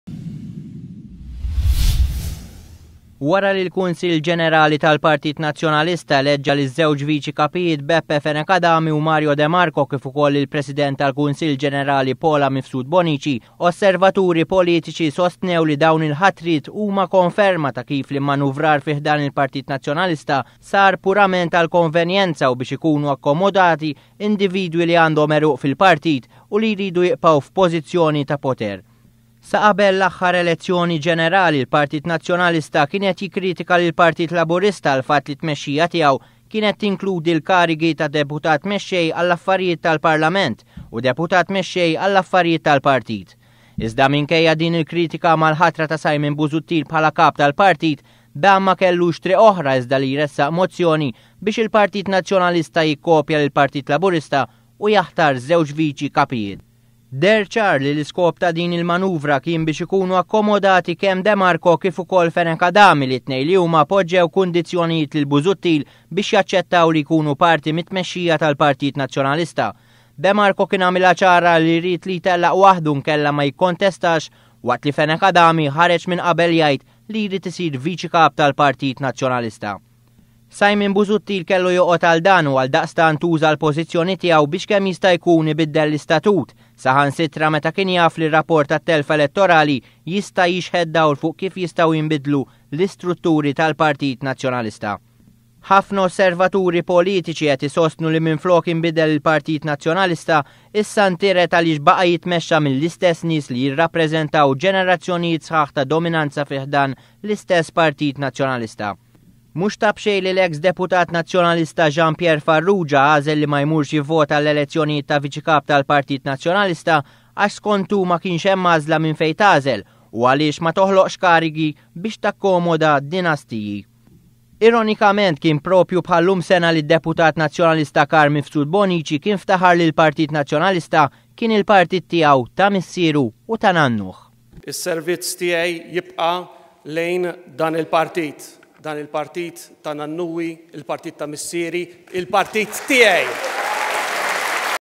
Il-ħatra taż-żewġ viċi-kapijiet tal-PN saru purament għal konvenjenza
Wara li l-Kunsill generali tal-partit nazzjonalista elegga liz-zewg vici-kapijiet Beppe Fenech Adami u Mario Demarco kif ukoll lill-President tal-Kunsill Generali Paula Mifsud Bonnici kienu hafna l-osservaturi politici  li sostnew li dawn il-hatriet saru purament ghal konvenjenza.